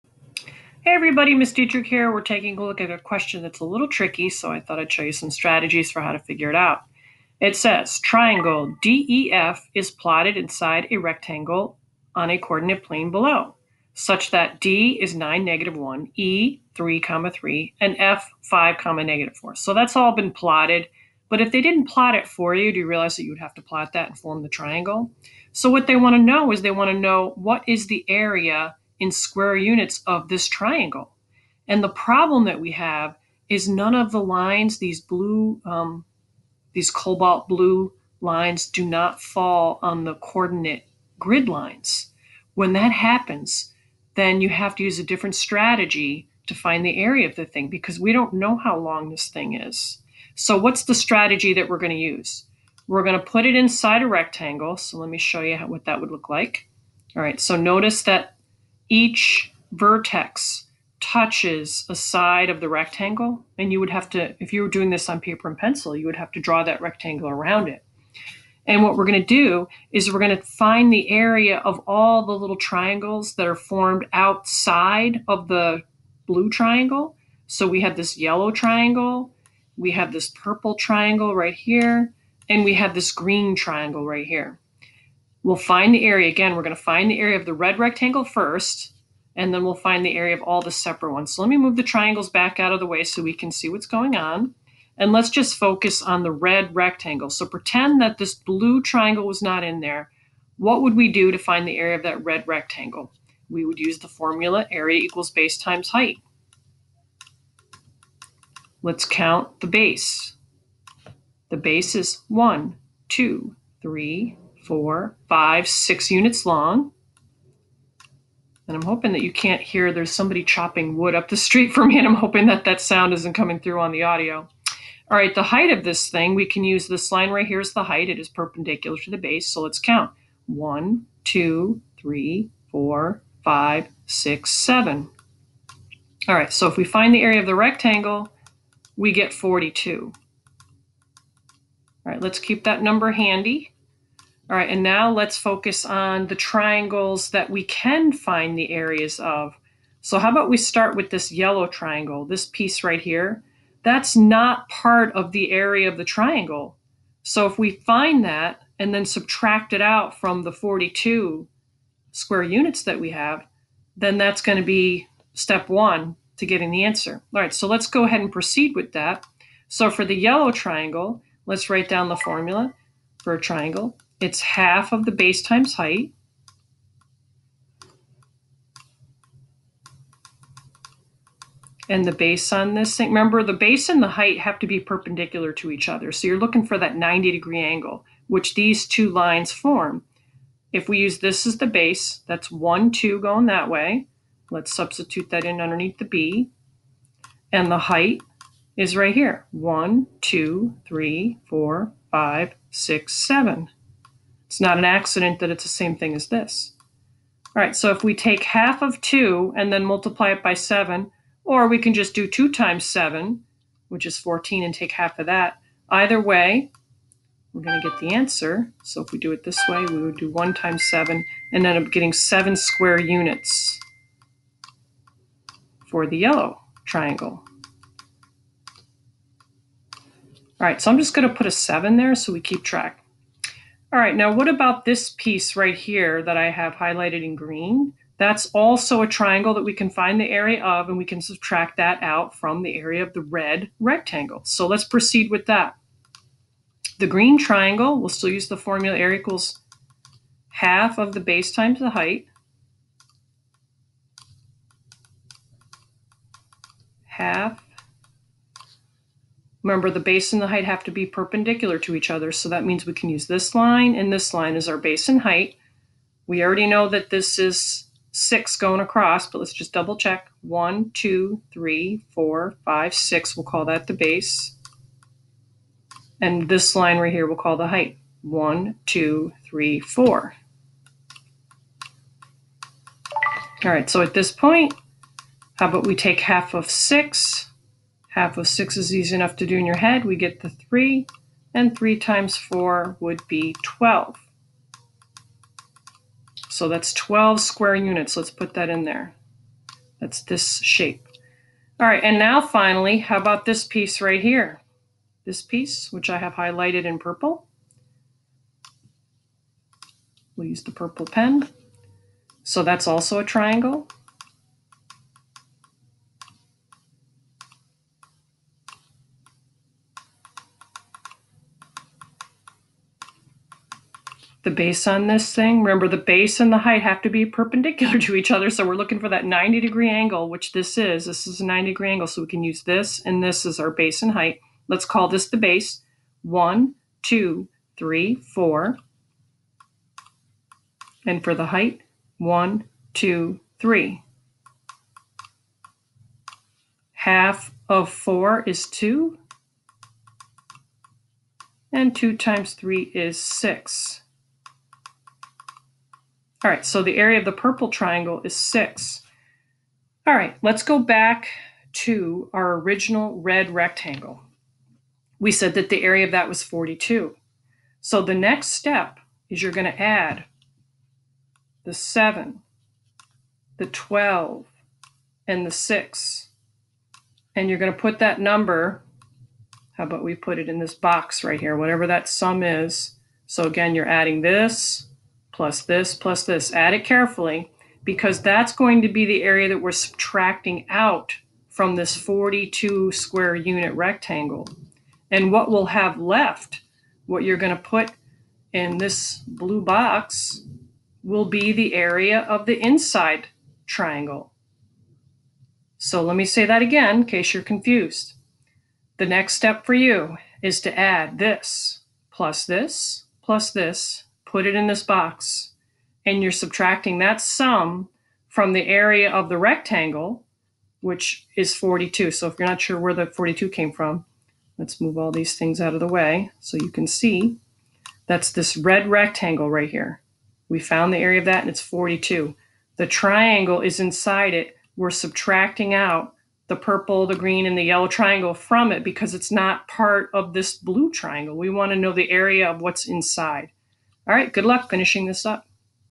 Finding the Area of Triangles on a Coordinate Plane
How do you find the area when none of the sides line up with the grid lines of the coordinate plane?